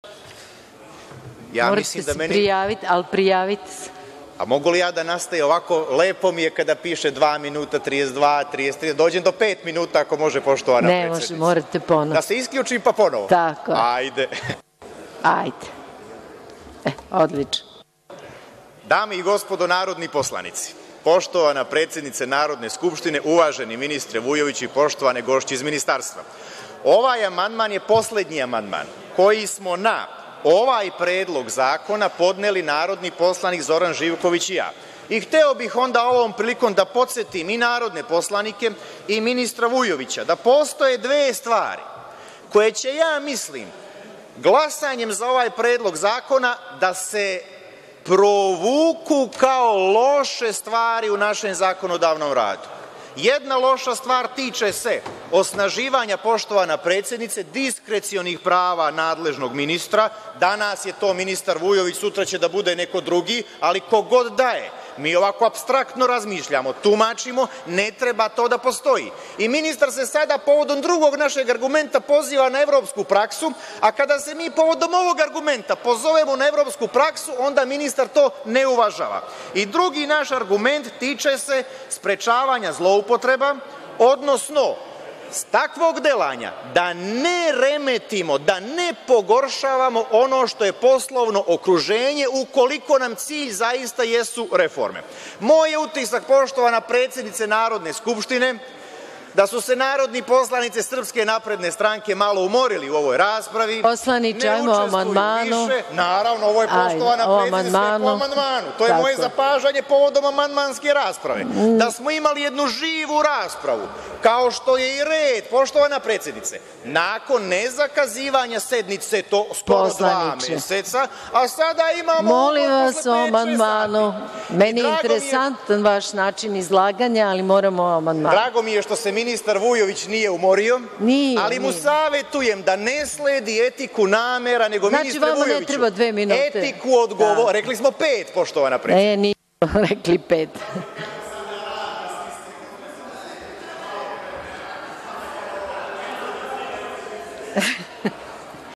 28.09.2015. - Pavićević: Moj utisak je da su se poslanici SNS malo umorili od rasprave danas!
Vladimir Pavićević, narodni poslanik i zamenik predsednika Nove stranke

Sednica: Sedamnaesto vanredno zasedanje Narodne skupštine Republike Srbije u 2015. godini